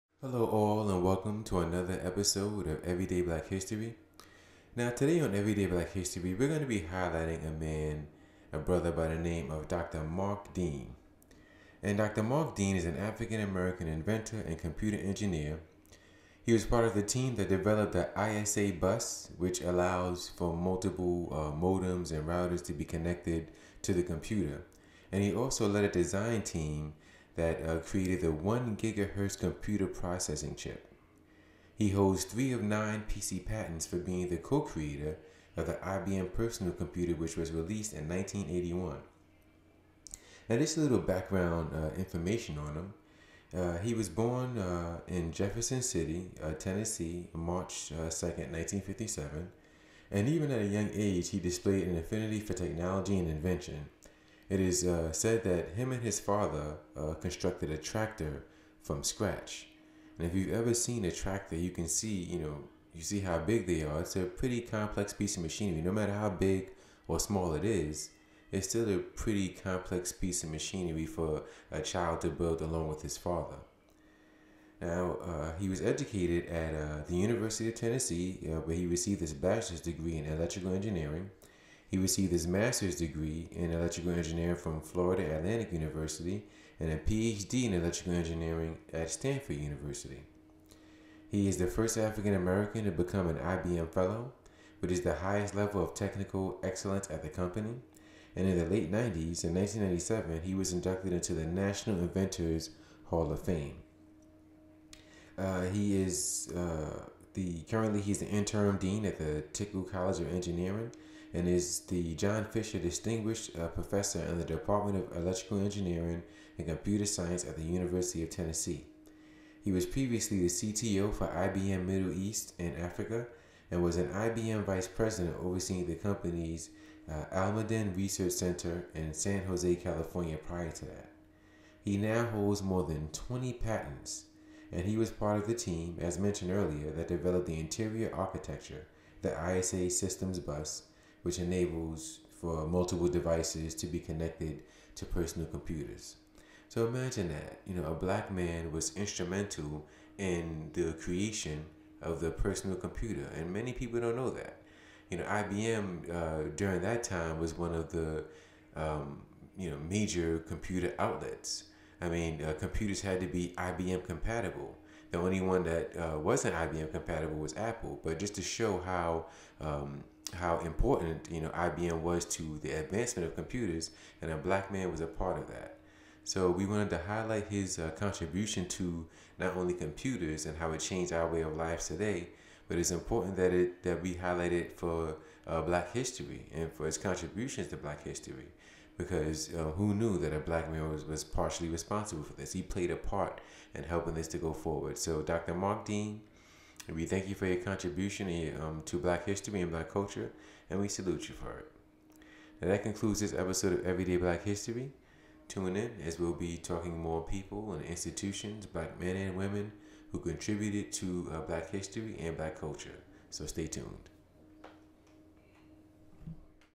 Dr. Mark Dean: One of the co creators of the IBM personal computer. Developed ISA bus for computer
This video is about Dr. Mark Dean, who is one of the co-creators of the IBM personal computer. He also led the design team for ISA bus which allows for multiple devices to connect to the computer as well as designing computer chips for the computer and many more. Please enjoy this video!